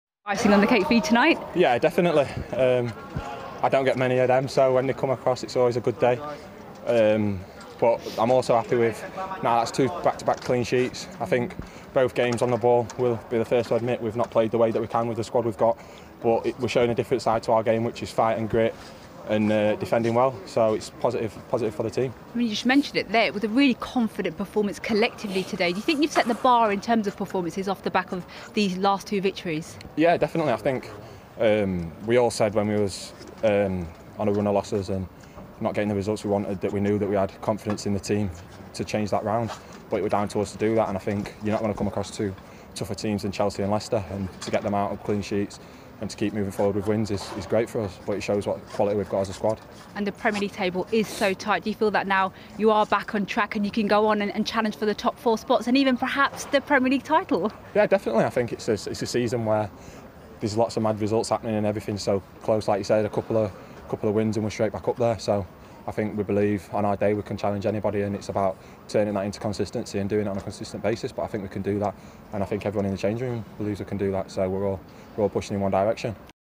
LEICESTER CITY 0-2 EVERTON | MASON HOLGATE'S REACTION
Mason Holgate scored his first Premier League goal to seal Everton's 2-0 win at Leicester City after Richarlison had given Carlo Ancelotti's Blues the lead.